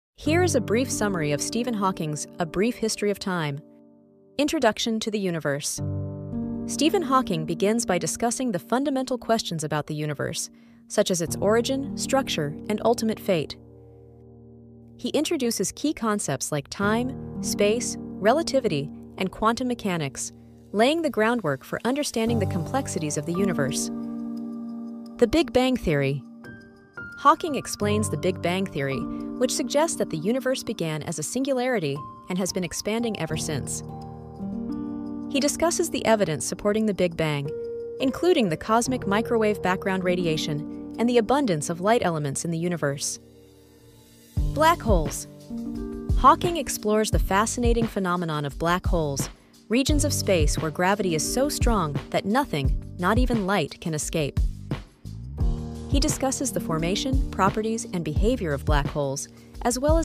A BRIEF HISTORY OF TIME
Description:
"Embark on a journey through time in this captivating YouTube short! From ancient sundials to modern atomic clocks, witness the evolution of timekeeping throughout history. Learn about pivotal moments in time measurement, from the invention of mechanical clocks to Einstein's theory of relativity. Explore how quantum clocks are revolutionizing our understanding of time itself. Join us as we unravel the mysteries of time in just a few minutes!